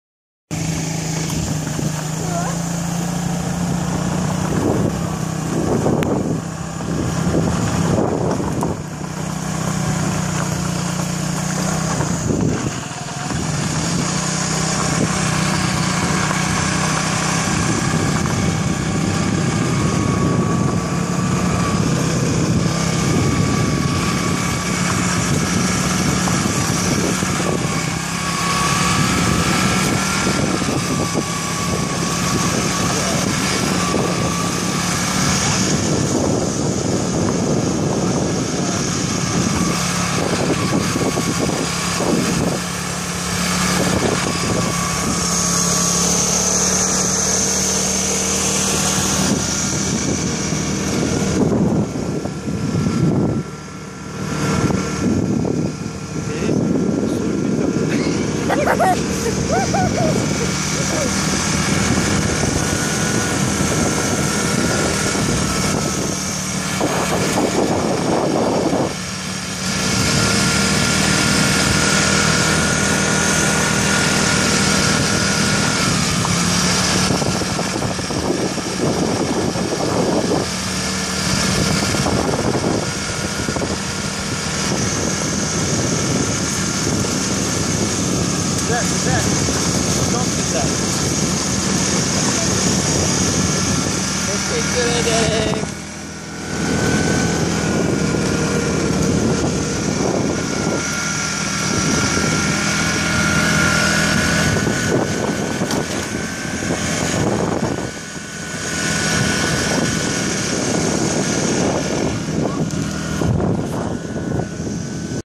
Shakir rides Aprilia Mojito 125 in Singapore off-roads
Shakir rides Aprilia Mojito 125 in Singapore off-roads, May 2013. Father sits behind and shoot the video !